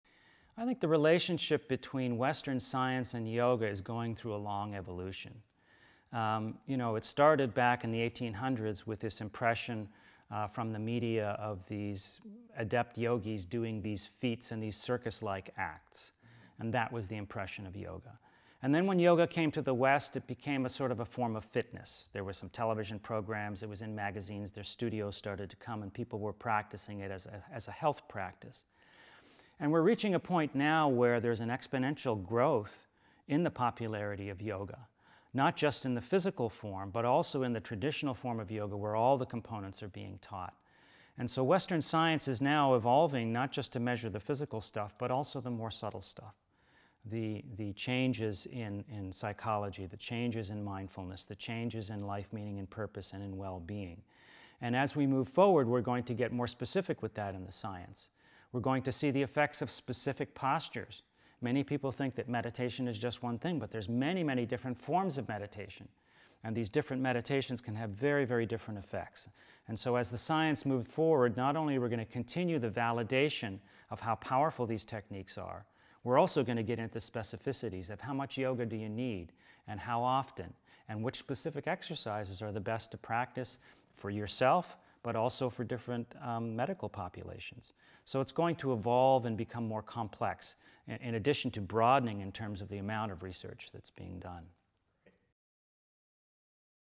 The Evolution of Science and Yoga with Sat Bir Singh Khalsa Ph.D
Sat Bir Singh Khalsa on what the Future holds for Science and Yoga.